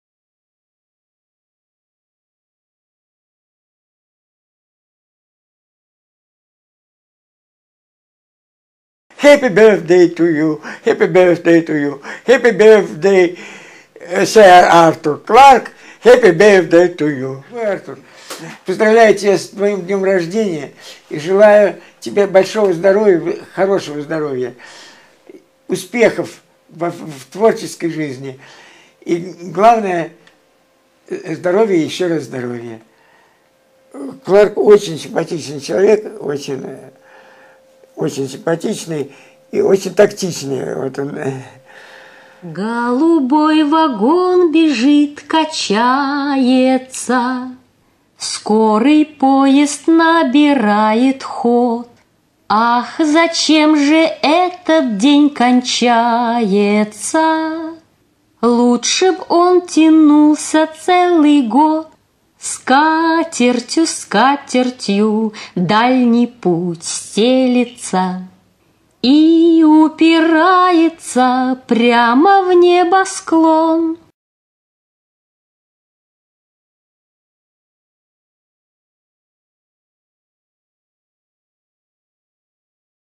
Yuri Artzutanov's Final Greeting to Arthur C. Clarke
Yuri N. Artzutanov's final birthday message to Sir Arthur C. Clarke.
Yuri was planning a visit to Arthur's home in the near future, but Arthur's departure only delays this meeting until some time - we hope for Yuri's sake- in the distant future when both can share a cup of tea made from the waters of Europa.

Yuri Artzutanov

A Russian visionary and engineer born in Leningrad, he is known for being one of the pioneers of the idea of the Space Elevator (a type of skyhook). In 1960 he wrote an article "V Kosmos na Electrovoze (English: Into Space with the Help of an Electric Locomotive)", where he discussed the concept of the space elevator as an economical, safe and convenient way to access orbit and facilitate space exploration.  The idea was popularized in "Fountains of Paradise" by Sir Arthur C. Clarke.